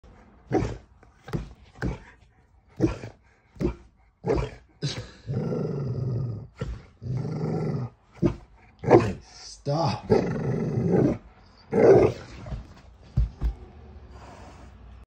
Crazy Dog and Cat Fight - Dog doesn’t want cat on bed
Crazy Dog Fight - Dog doesn’t want cat on bed. The Golden Retriever does not want the cat on the bed. The dog fights the cat to get him off. Typical dog being jealous wanting everything to herself. Picture your animal doing the same. It is a pretty funny video to watch